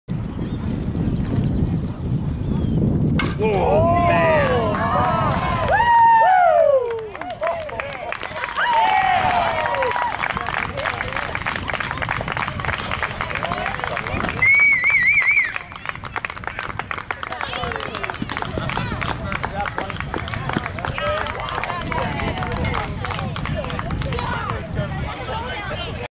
2017-April-19 - Dr Mack 8 vs Yoli 6 (Roderick Tanamor, Grand Slam HR)
Dr. Mack Orthodontics Wins Back-And-Forth Affair Against Yoli

Dr. Mack Orthodontics took the lead for good in the second inning to capture a back-and-forth game from Yoli by an 8-6 score on Tuesday. The game was tied at four with Dr. Mack Orthodontics batting in the top of the second when Roderick T hit a grand slam off of Alex M, driving in four runs.
Roderick T led Dr. Mack Orthodontics to victory by driving in four runs. He went 2-for-2 at the plate. Roderick drove in runs on a grand slam in the second.
Dr. Mack Orthodontics took an early lead in the first inning. A wild pitch by Parker B scored two runs for Dr. Mack Orthodontics.
Dr. Mack Orthodontics took the lead for good with five runs in the second inning. In the second Landon T singled off of Alex M, driving in one run.
Dr. Mack Orthodontics had seven hits in the game. Landon and Roderick each collected multiple hits for Dr. Mack Orthodontics. Landon led Dr. Mack Orthodontics with three hits in four at bats.
Yoli collected eight hits. Parker, Brycen A, and Dalton M each collected multiple hits for Yoli.

"Powered by Narrative Science and GameChanger Media. Any reuse or republication of this story must include the preceding attribution.